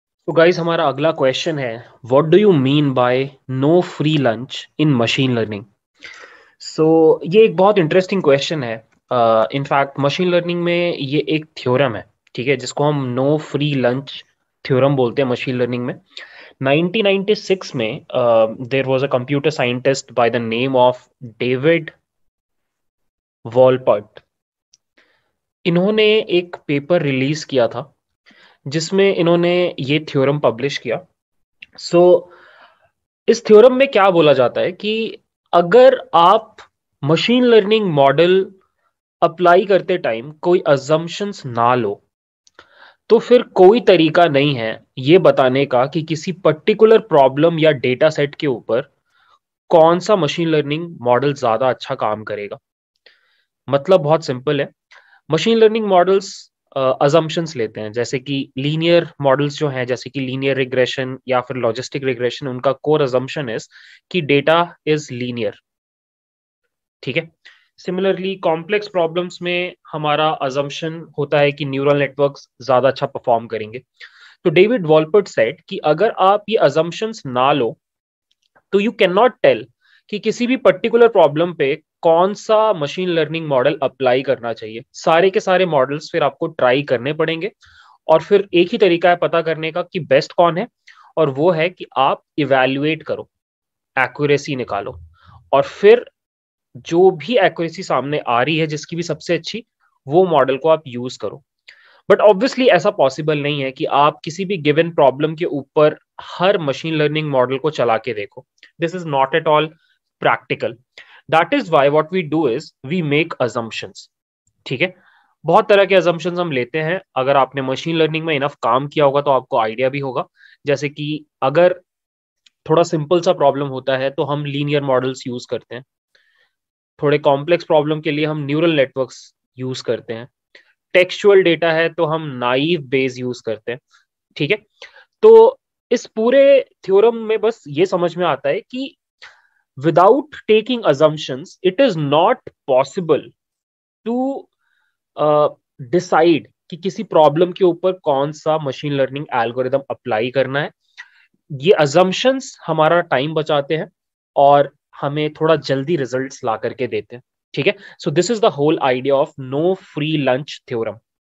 What is No Free Lunch Theorem in Machine Learning? | Interview Question
In this video, we talk about the "No Free Lunch" theorem in machine learning. We also discuss how to tackle the interview questions around this correctly and effectively.

Share your thoughts, experiences, or questions in the comments below. I love hearing from you!

Do you want to learn from me?